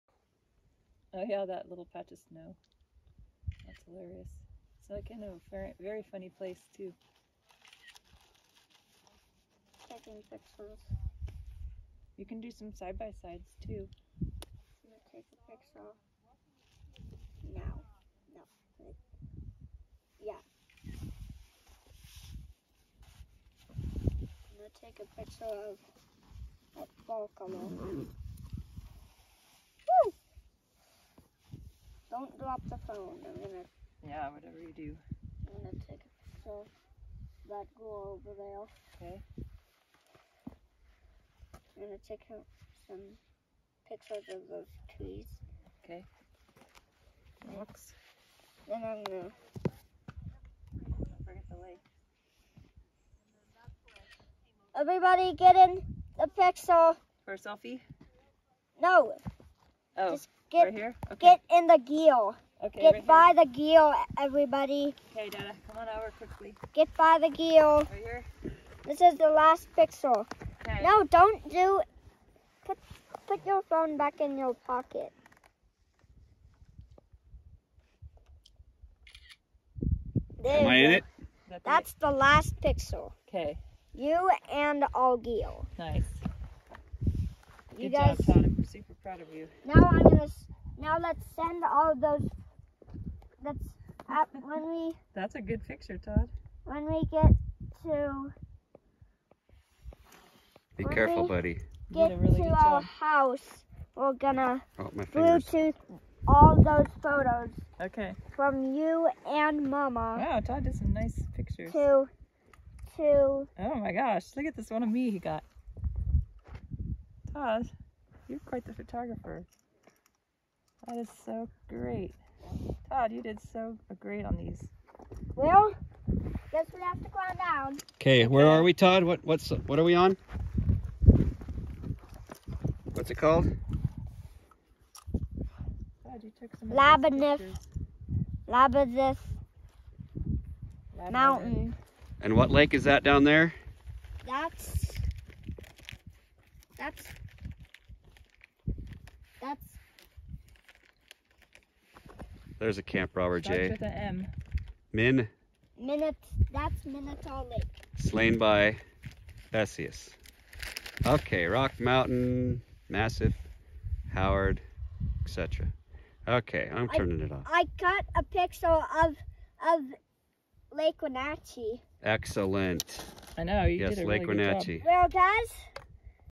At Labyrinth Mountain taking pictures of Minotaur Lake
IMG_2486.MOV - 10/03/21
Hiking near Lake Wenatchee, WA